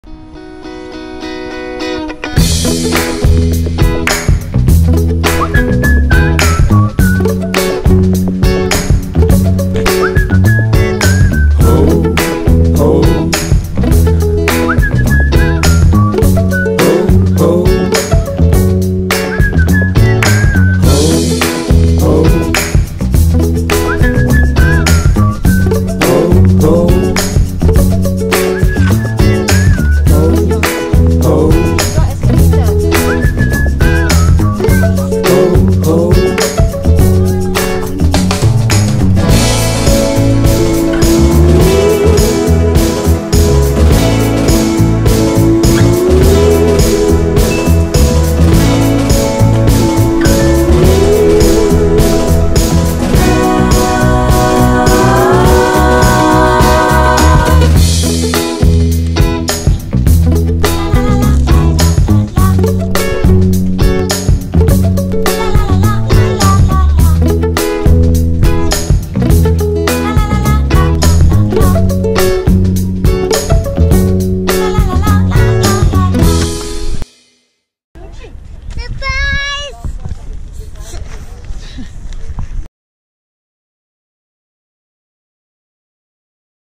Family Fishing @ Bedok Jetty !
To Yusuf:
Your father has never attempted fishing in his 33 years of life. It was only in 2020 that he, your Aunty Aqilah and Nenek Al attempted fishing. lol. We did not caught any big fishes but It was fun, Azka and you had a good time. Uncle Zul will bring us to Tioman once COVID-19 is overrrr.